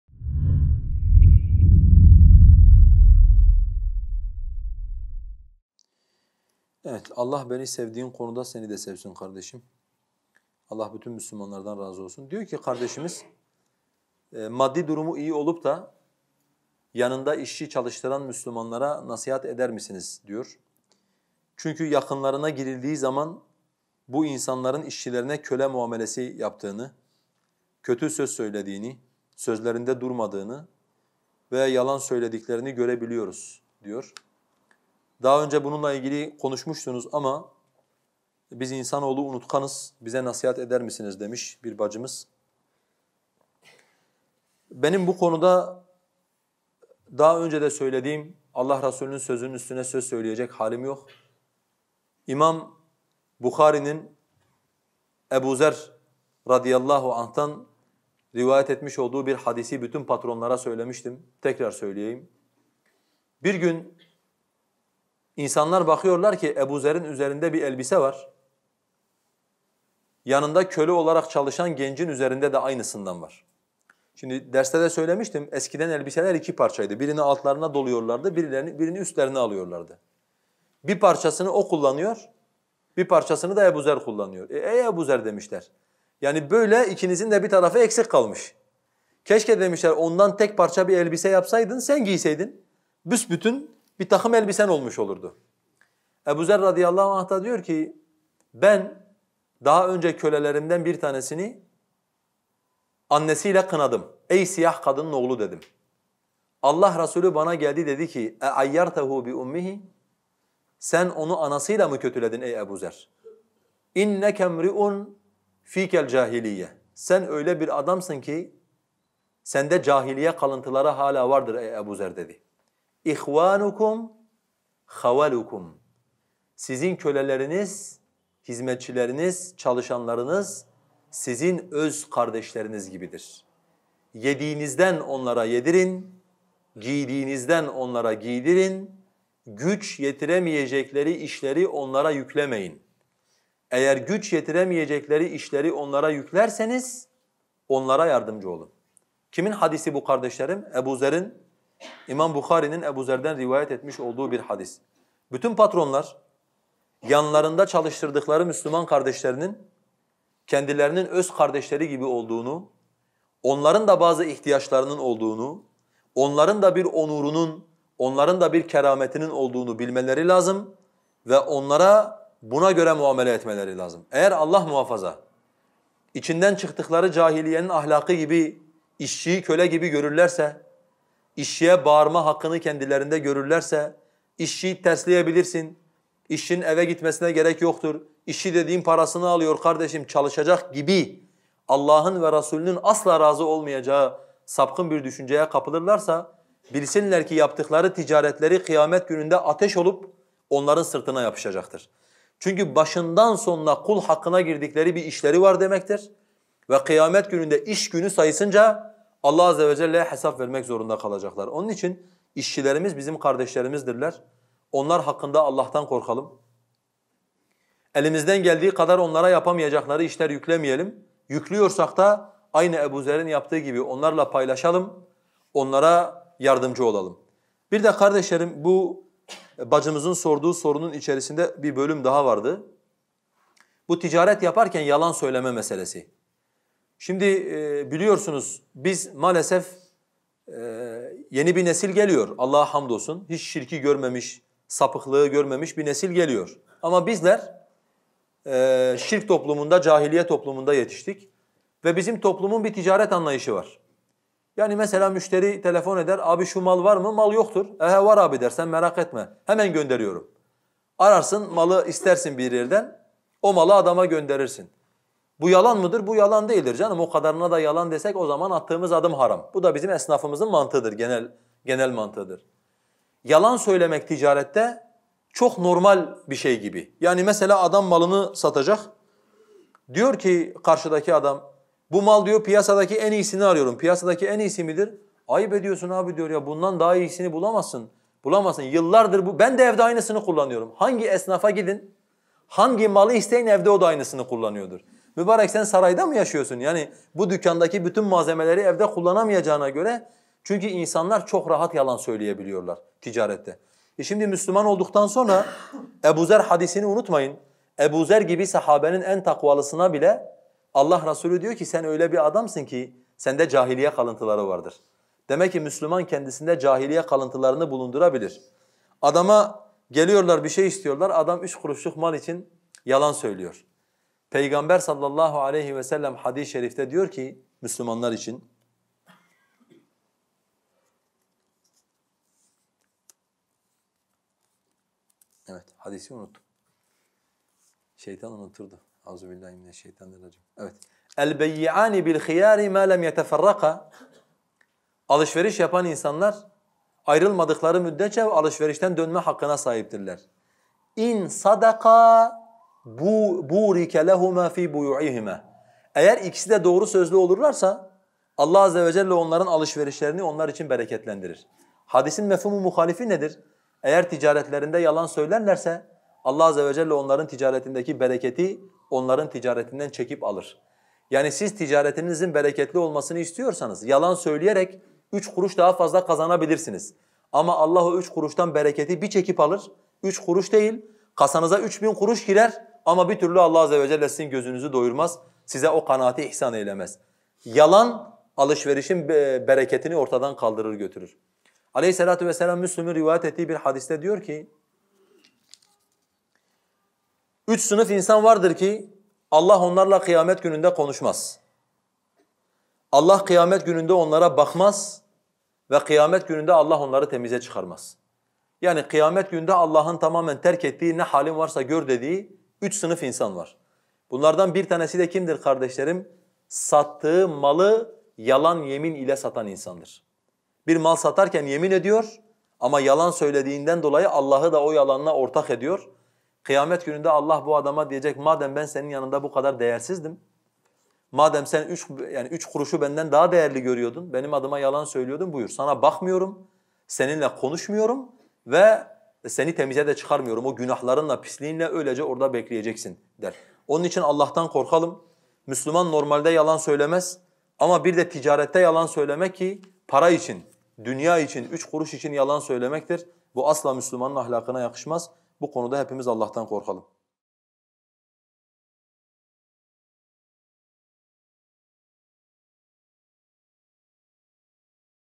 İşçilerine zulmeden Müslimlere nasihat eder misiniz? Halis Bayancuk Hoca
Soru:
İşçilerine zulmeden Müslimlere nasihat eder misiniz?

Cevap:
İnsanlar bakıyor ki ebuzerin üzerinden bir elbise var. Kölesinde de aynı elbiseden var. Eskiden elbiseler iki kısımdı. Birini altlarına doluyorlardı birini üstlerine giyiyorlardı. Bir parçasını köle bir parçasını ebu zer kullanıyordu. Ey Ebu Zer demişler böyle ikinizinde bir tarafı eksik kalmış. Keşke demişler ondan tekbir parça elbise yapsaydın sen giyseydin. Ebu Zer (rh) diyor ki: ben daha önce kölelerimden bir tanesini annesiyle kınadım, "Ey siyah kadının oğlu" dedim. Allah Resûlü geldi bana dedi ki: "Sen onu anasıylamı kötüledin ey Ebu Zer, sen öyle bir adamsın ki sende cahiliye kalıntıları hala vardır ey Ebu Zer." dedi. "Sizin köleleriniz, hizmetçileriniz, çalışanlarınız sizin öz kardeşleriniz gibidir. Yediğinizden onlara yedirin, giydiğinizden onlara giydirin, güç yetiremeyecekleri işleri onlara yüklemeyin. Eğer güç yetiremeyecekleri yükleri onlara yüklerseniz onlara yardımcı olun. (Buhari)

Bütün patronlar yanlarında çalıştırdıkları müslüman kardeşlerinin kendilerinin öz kardeşleri gibi olduğunu, onlarında bazı ihtiyaçlarının olduğunu, onlarında bir onurunun olduğunu, onlarında bir kerametinin olduğunu bilmeleri lazım ve onlara buna göre muamele etmeleri lazım. Eğer Allah muhafaza içinden çıktıkları cahiliyenin ahlakı gibi işçiyi köle gibi görürlerse, işçiye bağırma hakkını kendilerinde görürlerse, işçiyi terleyebilirsin, işçinin eve gitmesine gerek yoktur, işçi dediğin parasını alıyor kardeşim çalışacak gibi Allah'ın ve Resûlünün asla razı olmayacağı sapkın bir düşünceye kapılırlarsa, bilsinler ki yaptıkları ticaret kıyamet gününde ateş olup onların sırtına yapışacaktır.

Çünkü başından sonuna kul hakkına girdikleri bir işleri var demektir ve kıyamet gününde iş günü sayısınca Allah!a (cc) hesap vermek zorunda kalacaklar demektir. Onun için işçilerimiz bizim kardeşlerimizdirler. onların hakkında Allah'tan korkalım, elimizden geldiği kadar onların yapamayacakları işleri onların üzerine yüklemeyelim, yükleyecek isek eğer yardım edelim.

Ebu Hanzala Hoca
Tevhid Dersleri
Tevhid Dergisi
Dini videolar
Halis Bayancuk